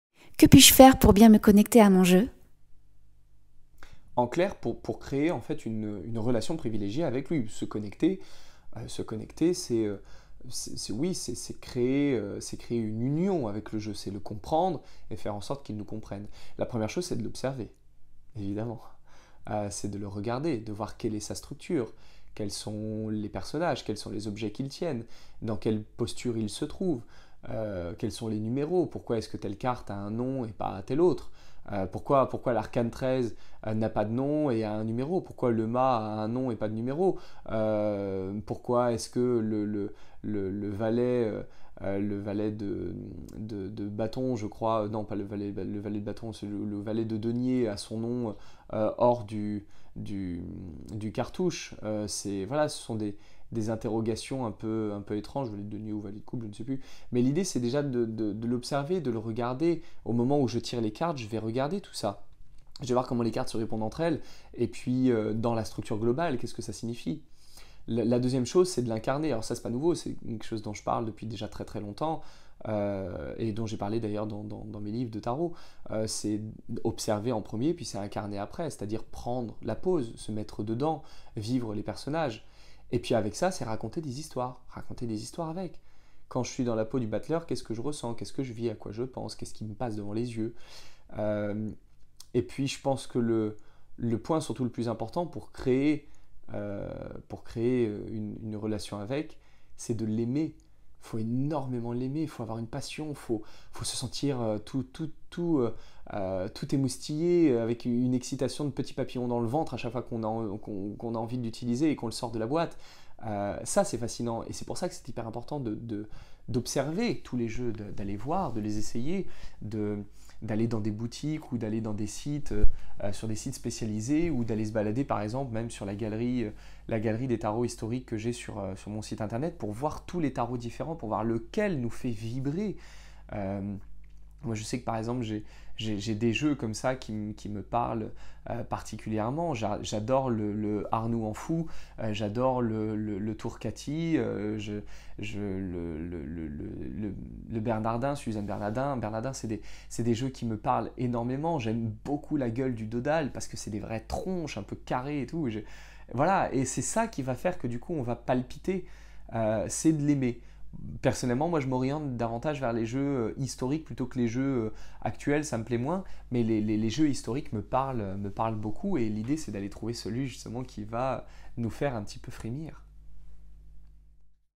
🎓 [Le Tarot en 24 Questions] 11- Que puis-je faire pour bien me connecter à mon jeu ?
Et si on se parlait un peu de tarot vous et moi ? Quelques questions en vrac parmi celles qui reviennent le plus souvent sur internet ou en interview, posées ici par Hélène Buannic. Merci à elle :)